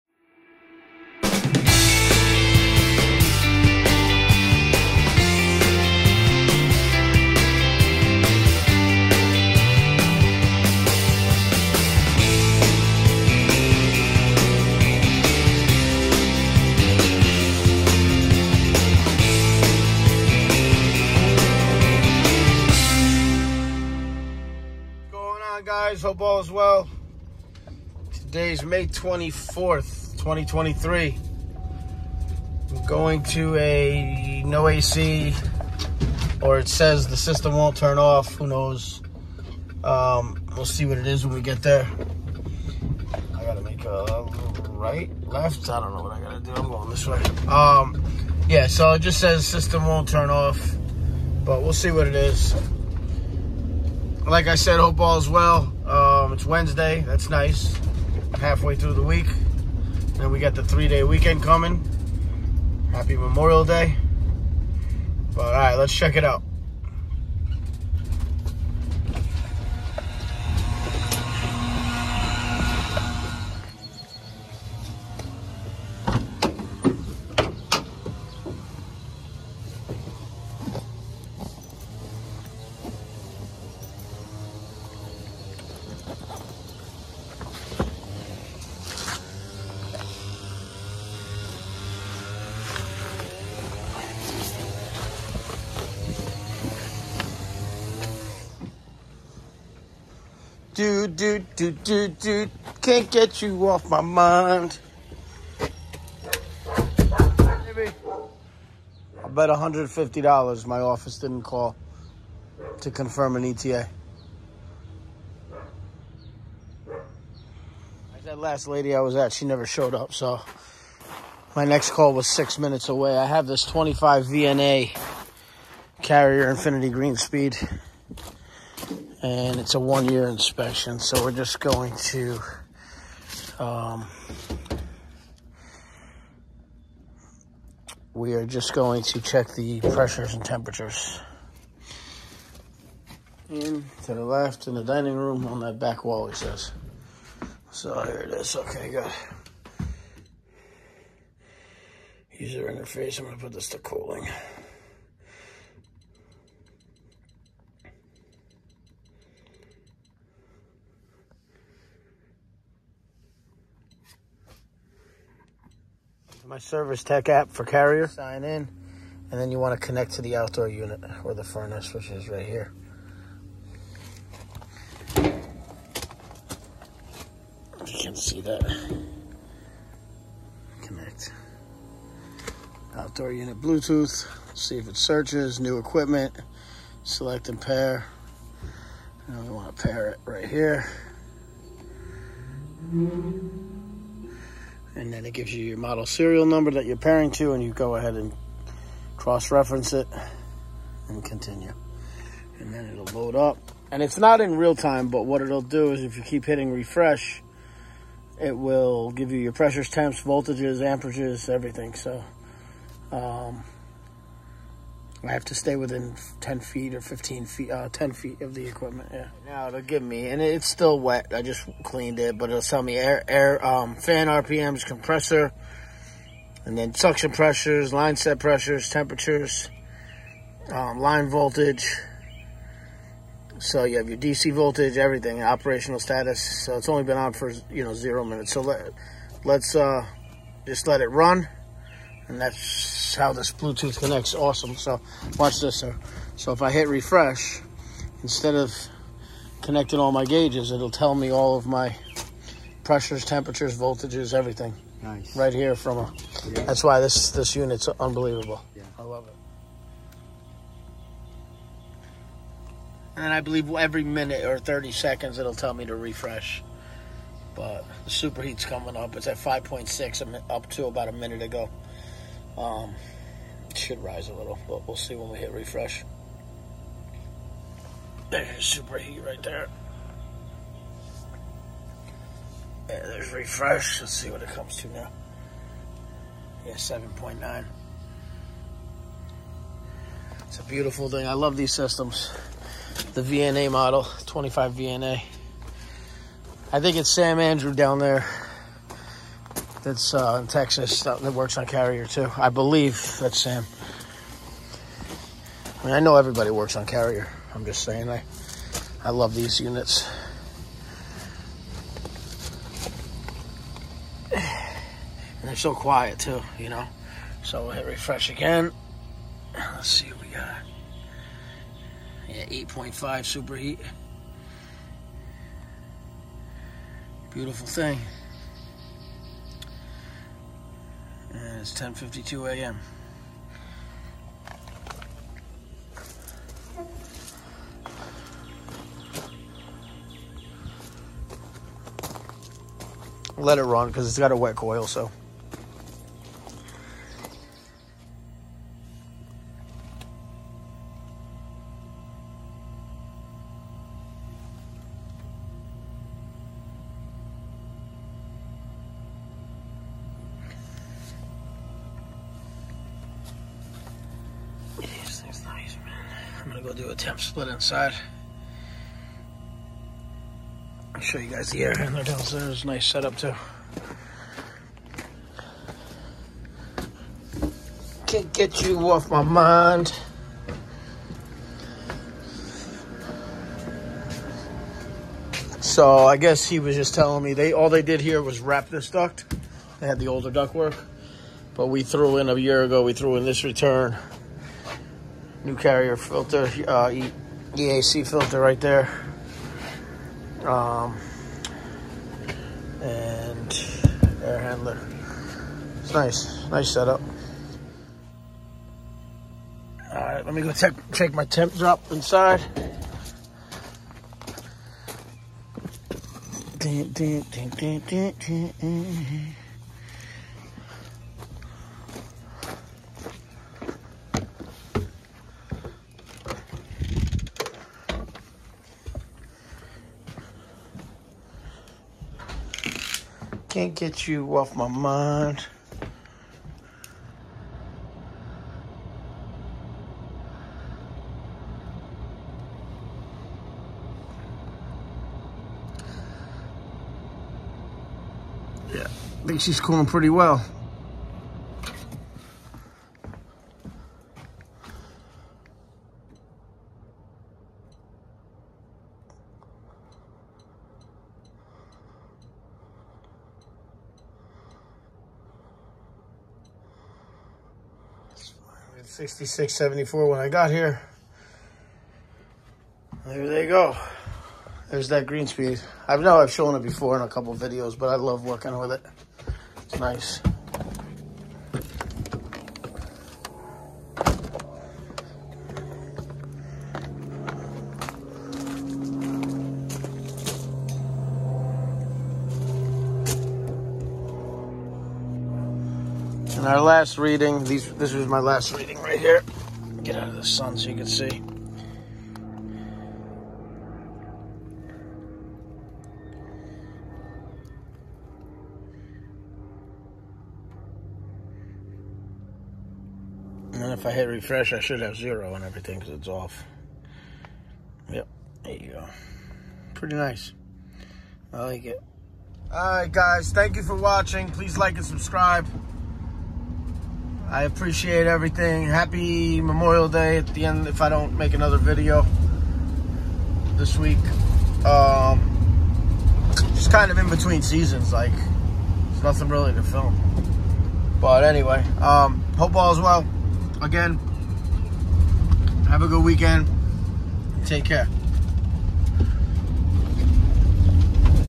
CARRIER INFINITY GREENSPEED 25VNA. BLUETOOTH.
Checking Carrier Infinity Green Speed split system.  25VNA model.   Bluetooth using Carrier Service Tech application